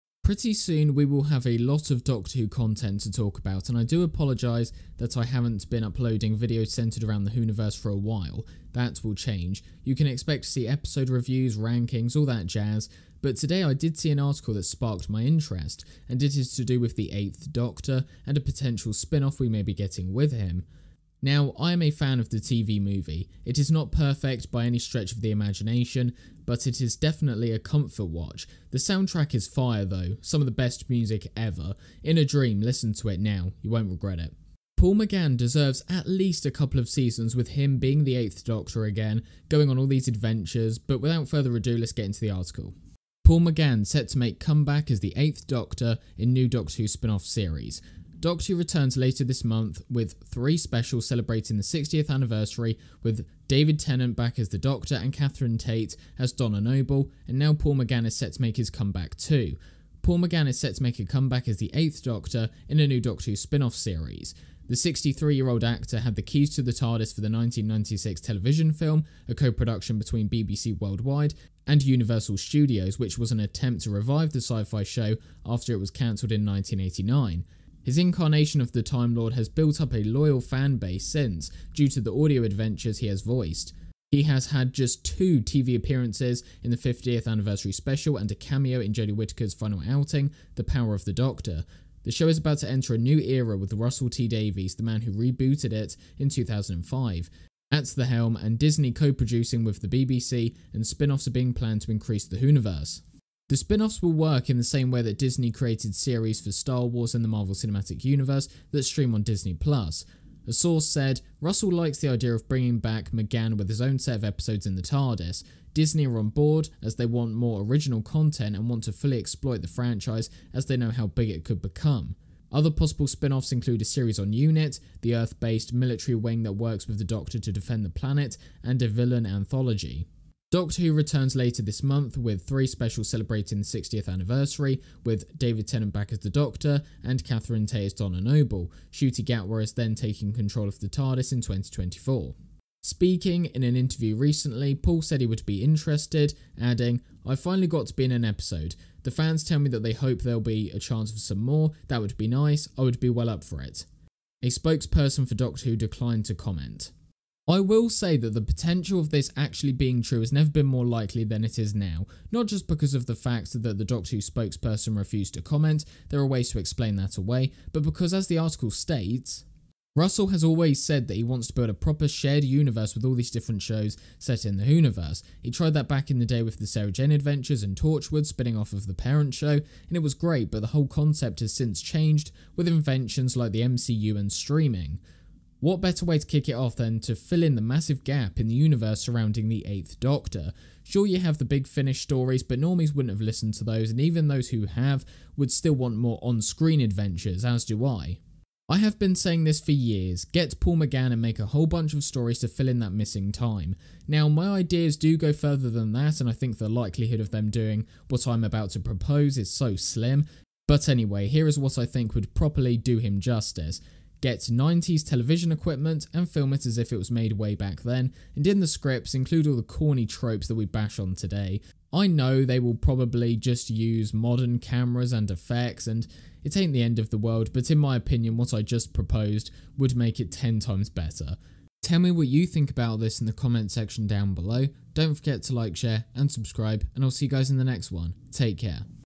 Eighth Doctor Set To RETURN In *NEW* Doctor Who Spin-Off Series! | Justice For Paul McGann!
Paul McGann is a much-beloved actor who is a key part of the Doctor Who universe despite having an extremely limited number of on-screen appearances.

The Eighth Doctor has been in many audio adventures and books but never had the chance to have his own seasons of the iconic show.

However, all of that appears to be changing because apparently Russell T Davies and Disney want to bring him back as part of an upcoming spin-off series.